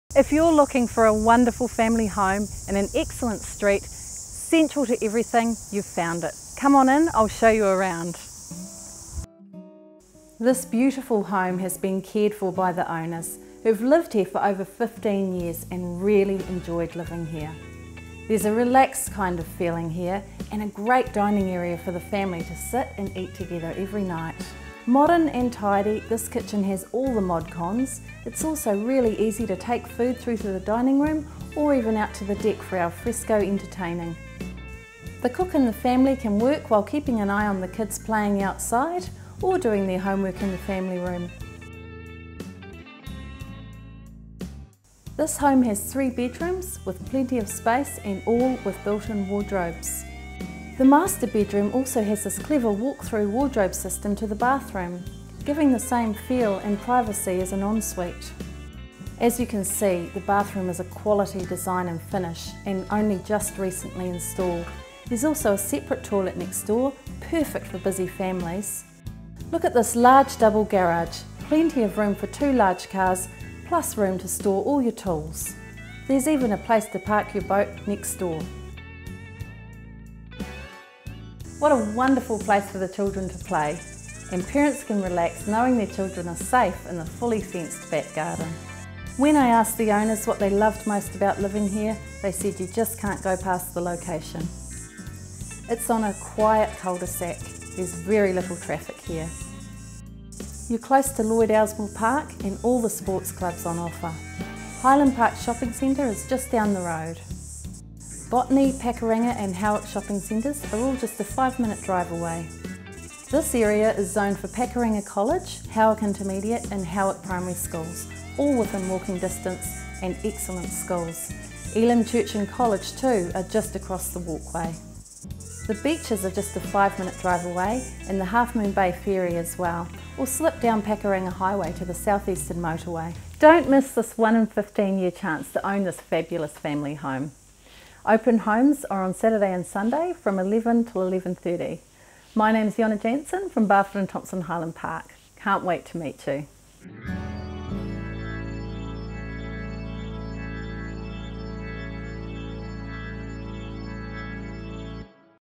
SOLD - 17 Coubray Place, Botany Downs -
This beautifully kept home on a quiet cul-de-sac, central to everything, will tick all your boxes!

Fully fenced and private, this delightful family home is located in a quiet cul-de-sac in the ...

Property Marketed by: